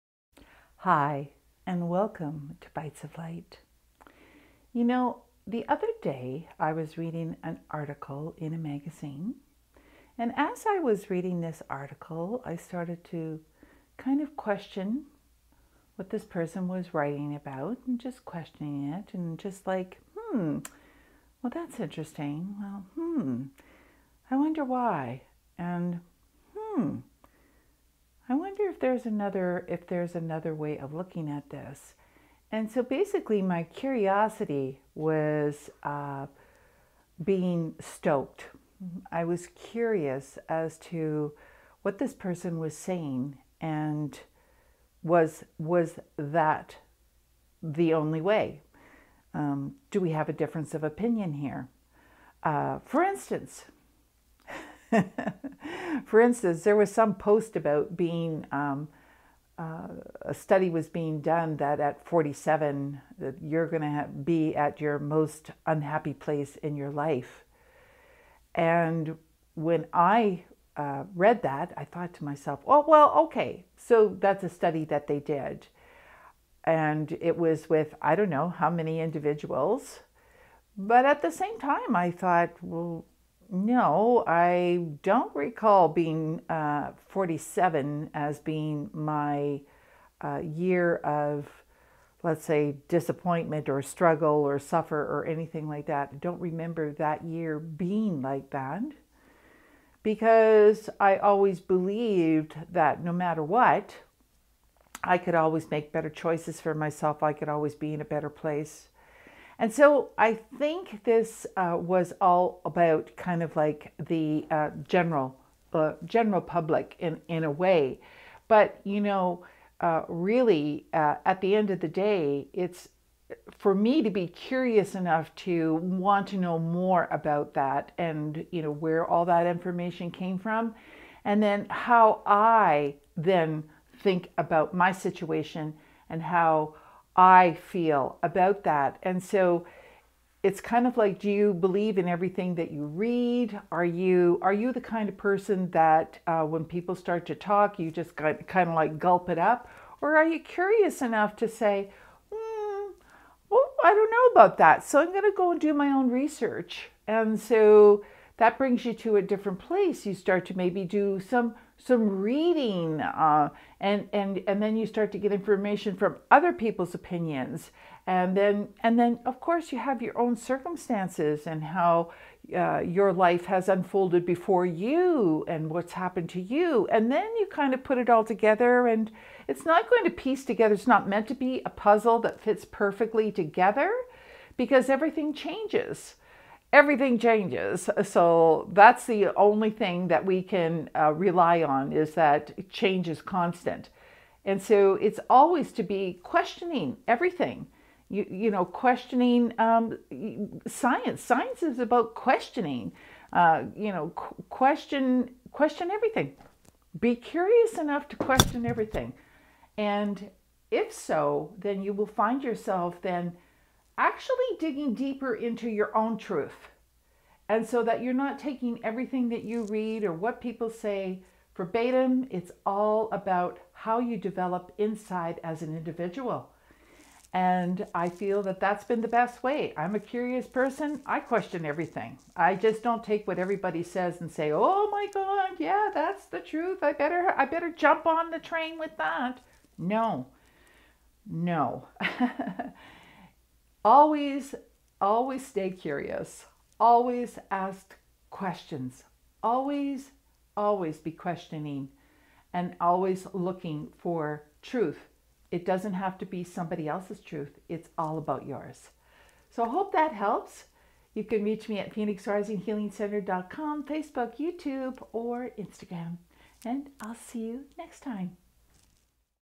Bites of Light: Ep. #174 - Be Curious and Open Minded
When new information or thoughts cross your path, what is your first instinct? Do you accept them for what they are, or are you likely to be a little bit more curious?
Do you do some research of your own?
While this Bite of Light might be a little bit late for the toilet paper industry 🤔😅😅😅 Adele encourages you to dig a little deeper. Be open minded and curious!

Learn more about Adele and the wide range of services she offers